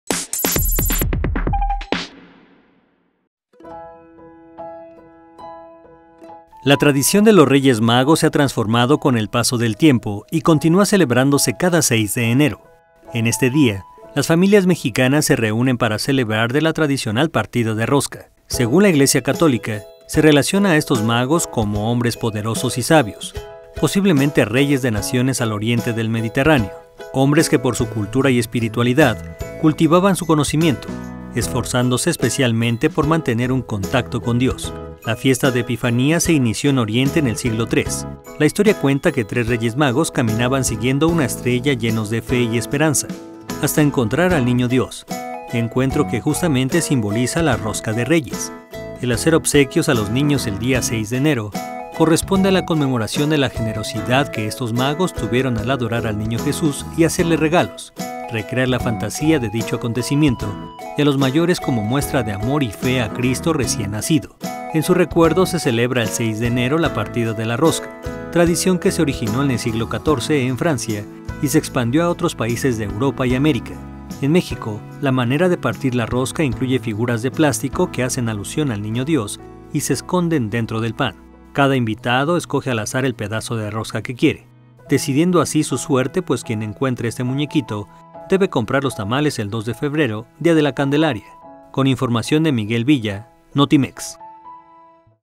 Día de Reyes tradición que continúa año con año
La tradición de los reyes magos se ha transformado con el paso del tiempo y continúa celebrándose cada 6 de enero. En este día, las familias mexicanas se reúnen para celebrar de la tradicional partida de rosca.